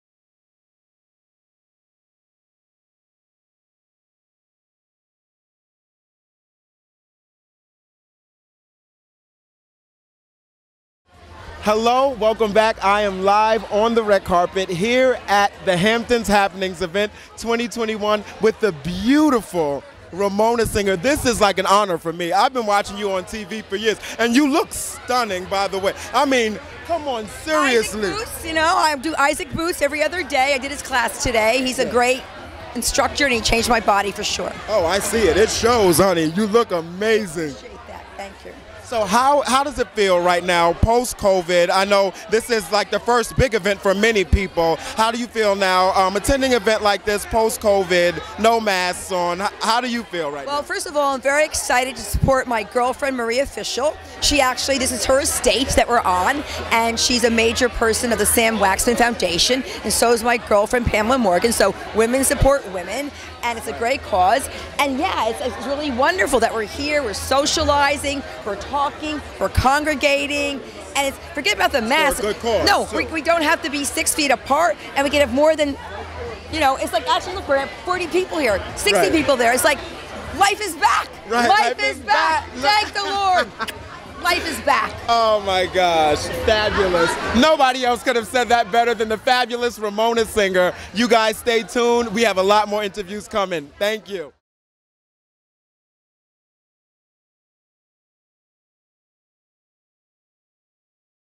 Behind the scenes with Shariff Sinclair interviews “REAL HOUSEWIVES OF NEW YORK” star Ramona Singer
An awesome interview with Ramona singer as she shows support to the Samuel Waxman Cancer research foundation.